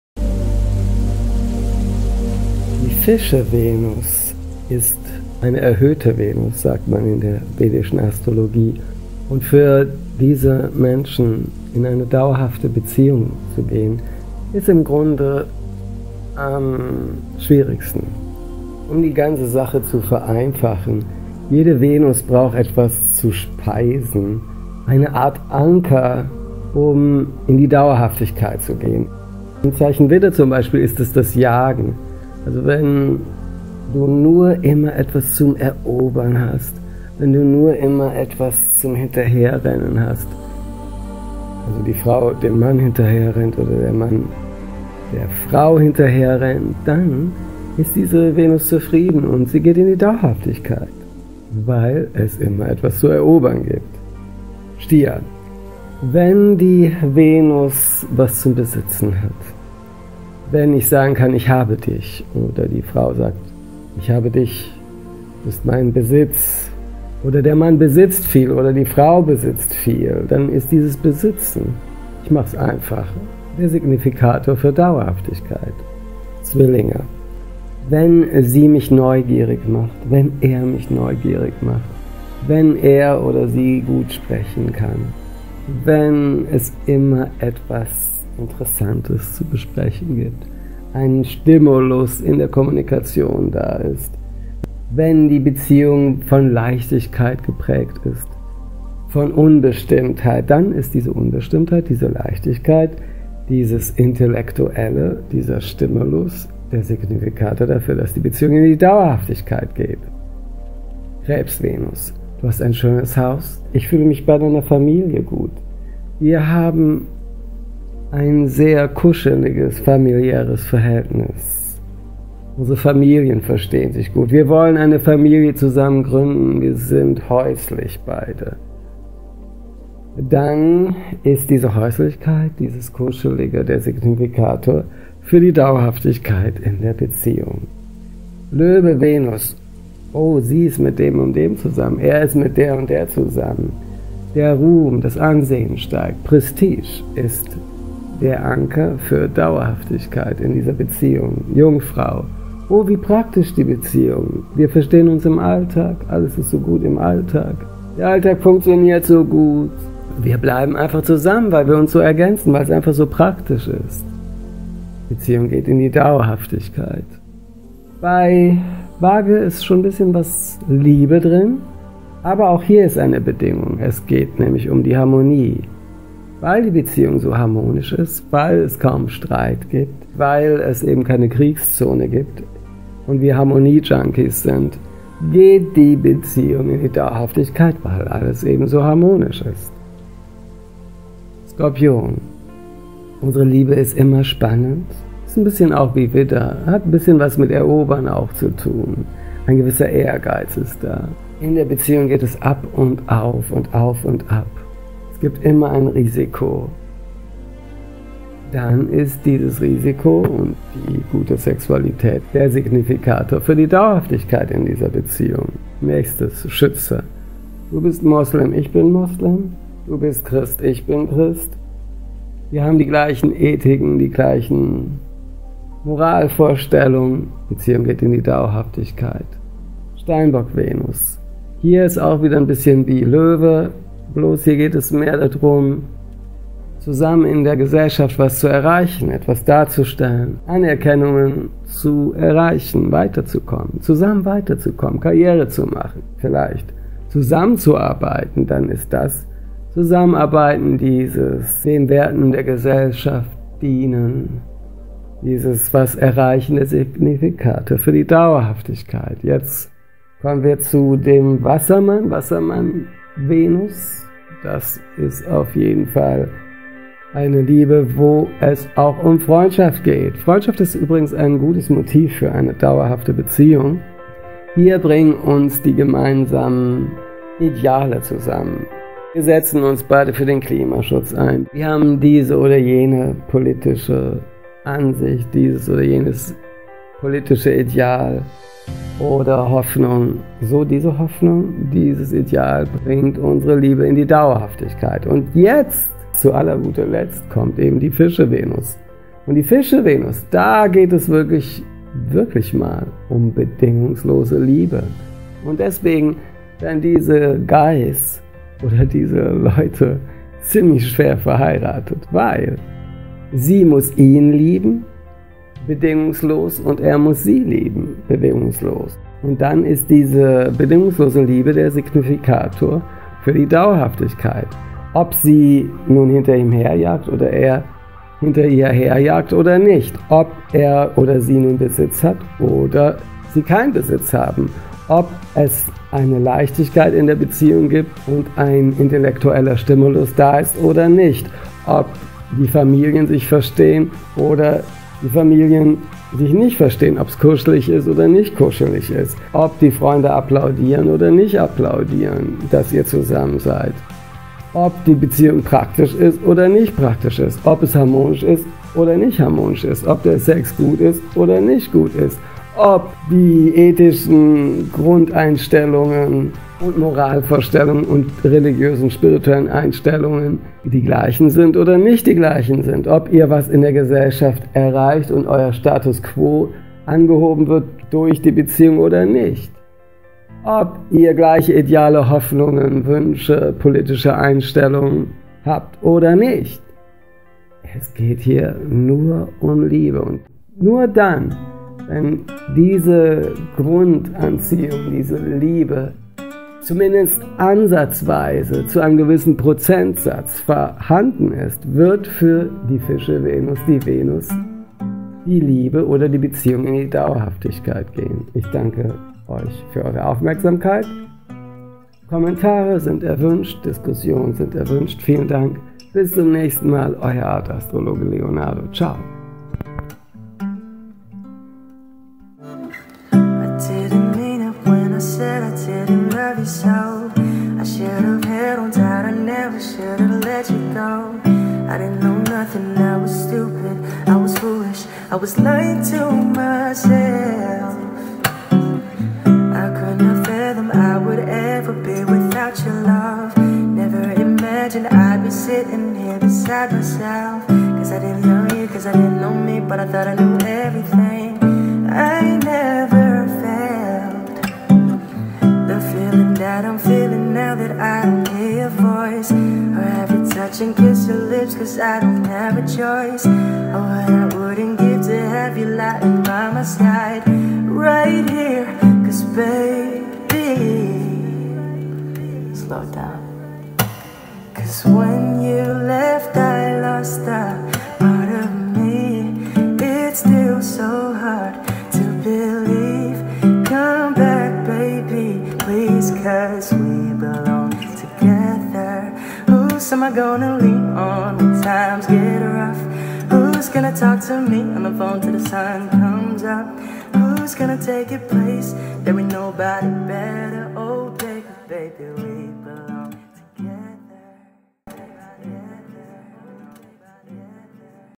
VENUS in FISCHE, die höchste, bedingungslose Liebe (eine Meditation)
Leute mit VENUS im FISCHE-Zeichen, tragen einen FLUCH mit sich, der gleichzeitig ein SEGEN ist: Das Schicksal wird Ihnen nur  erlauben mit jmd. in eine dauerhafte Beziehung zu gehen, wenn SIE die Person BEDINGUNGSLOS lieben und die Person sie bedingungslos liebt  Ich gehe durch alle anderen 11 Radix-Zeichen, um heraus-zu-arbeiten, wie die FISCHE-Venus im Grunde die einzige unmotivierte, bedingungslose Liebe ist.